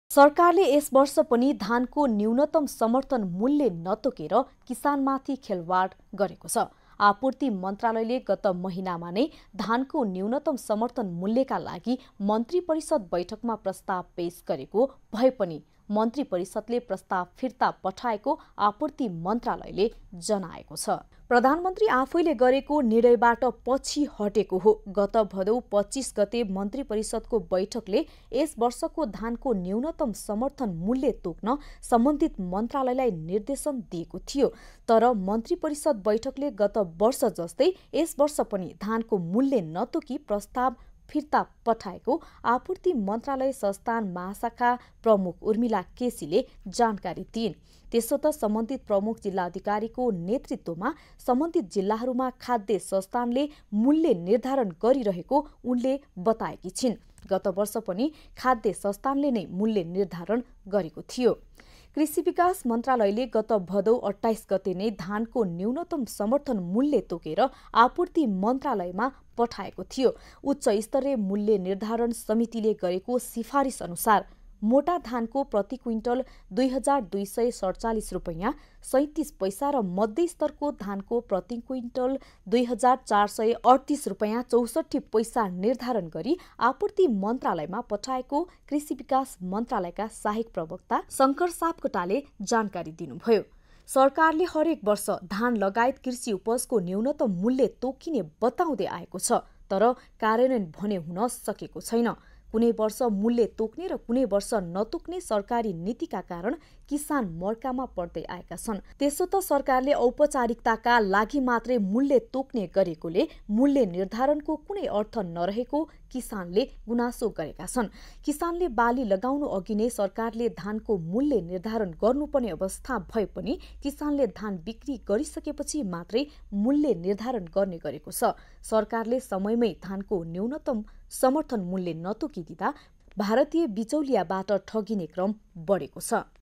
किसानमाथि किन खेलवाड ?
सरकारले यसवर्ष पनि धानको न्यूनतम समर्थन मूल्य नतोकेर किसानमाथि खेलवाड गरेको छ । आपूर्ति मन्त्रालयले गत महिना नै धानको न्यूनतम समर्थन मूल्यका लागि मन्त्रिपरिषद् बैठकमा प्रस्ताव पेस गरेको भएपनि मन्त्रिपरिषद्ले प्रस्ताव फिर्ता पठाएको आपूर्ति मन्त्रालयले जनाएको छ ।